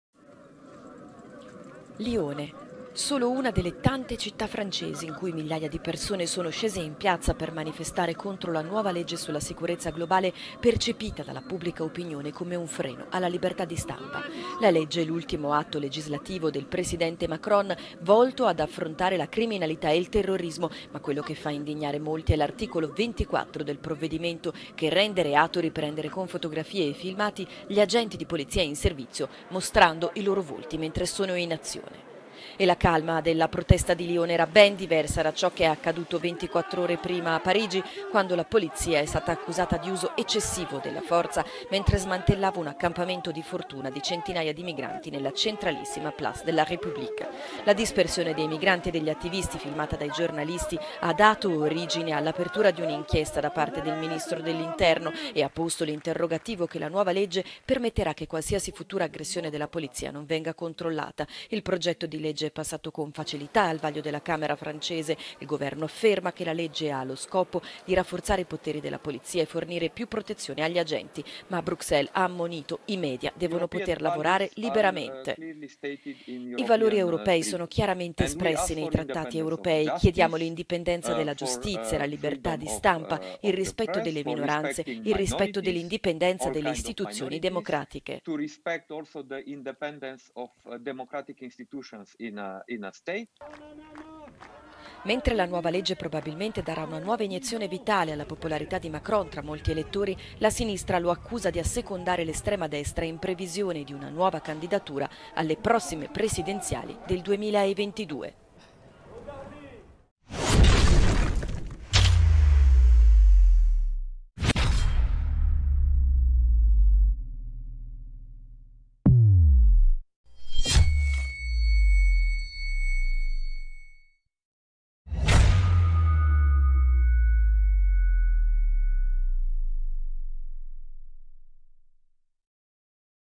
✅ SIONISMO IN FRANCIA Passa la legge sulla Sicurezza Globale
✅ OPPRESSIONE GOVERNATIVA SIONISTA IN FRANCIA. Il governo francese è a capofila del Sionismo che avanza in Europa. Passa la legge sulla "sicurezza globale", proteste di piazza e i media di regime ne confermano la natura.

📌 MISTIFICAZIONI RACCONTATE DAI MEDIA SULLA LEGGE EMESSA: Una minaccia alla libertà di stampa. Lione. Solo una delle tante città francesi in cui migliaia di persone sono scese in piazza per manifestare contro la nuova legge sulla sicurezza globale, percepita dalla pubblica opinione come un freno alla libertà di stampa.
La legge è l'ultimo atto legislativo del presidente Macron volto ad affrontare la criminalità e il terrorismo. Ma quello che fa indignare molti è l'articolo 24 del provvedimento che rende reato riprendere con fotografie e filmati gli agenti di polizia in servizio, mostrando i loro volti mentre sono in azione.

by:GR Channel